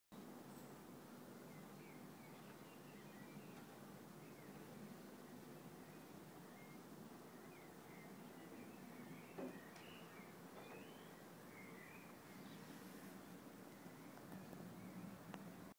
Frankfurter Tor from warschauer61 roof terrace
Small video showing the towers of Frankfurter Tor from the roof terrace of the warschauer61 apartment in Berlin, Friedrichshain.